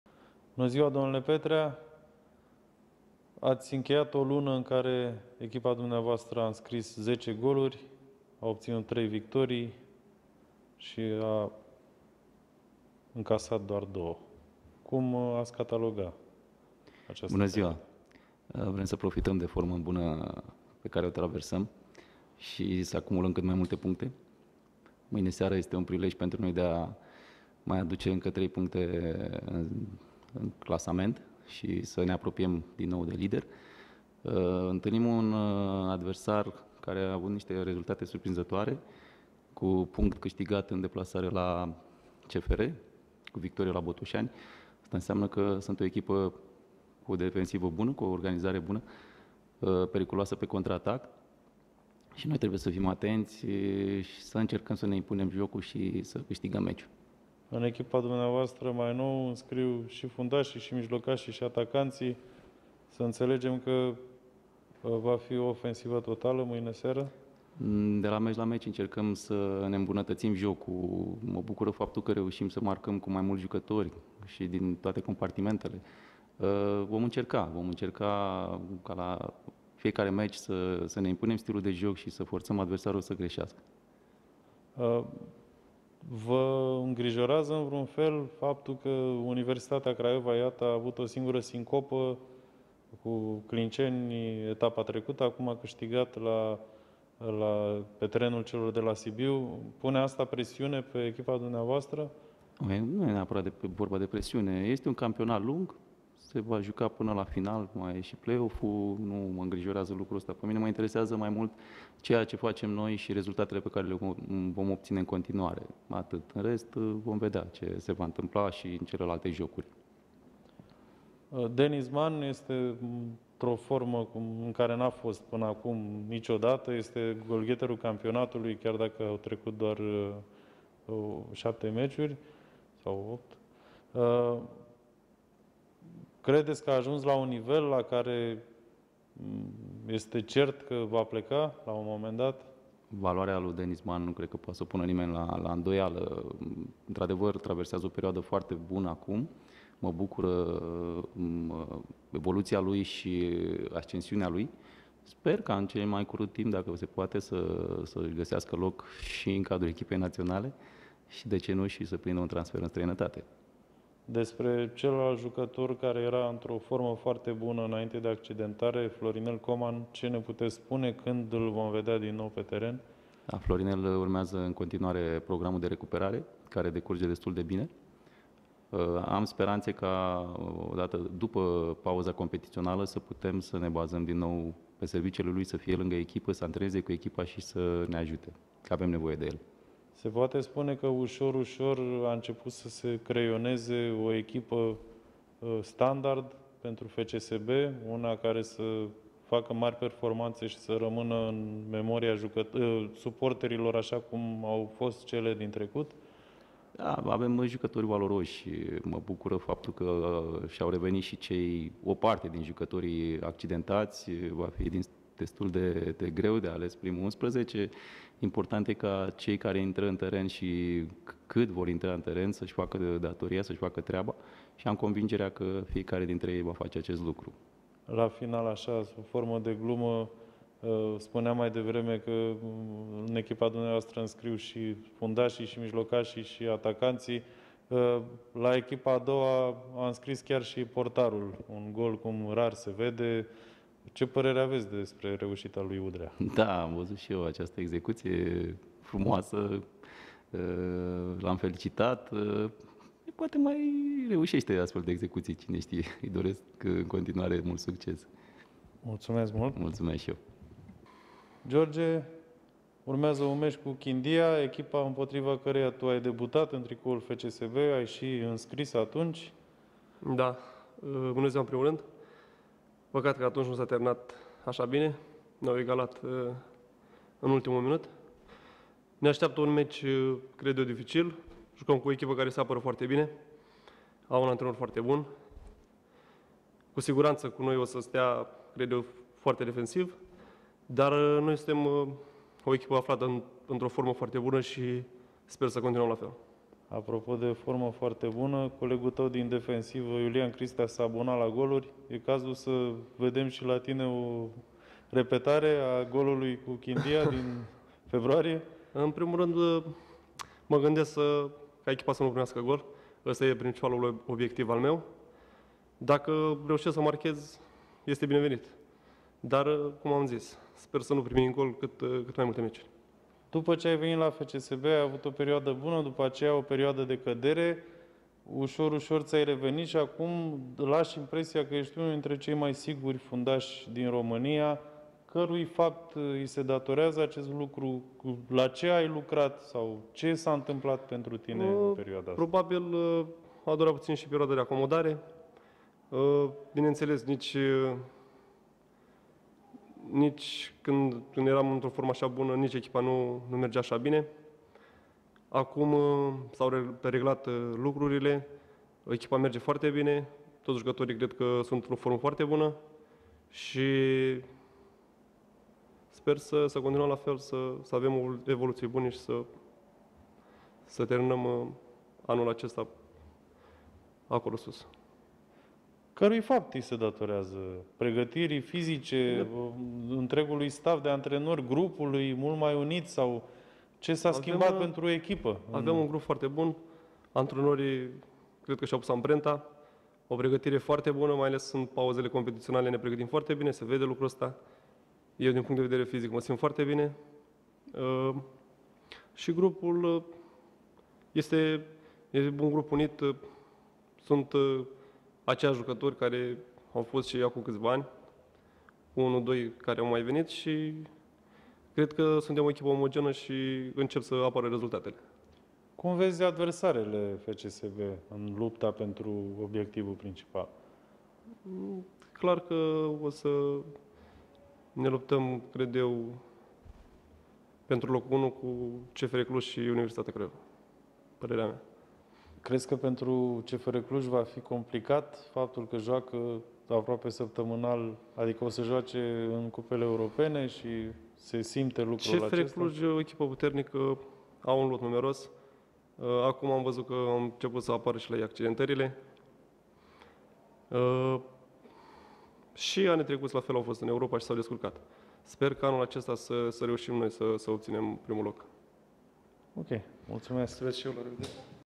Conferință de presă Anton Petrea și George Miron!
Aici puteți vedea ce au declarat Anton Petrea și George Miron înaintea partidei cu FC Chindia Târgovişte, din etapa a 9-a a Ligii 1.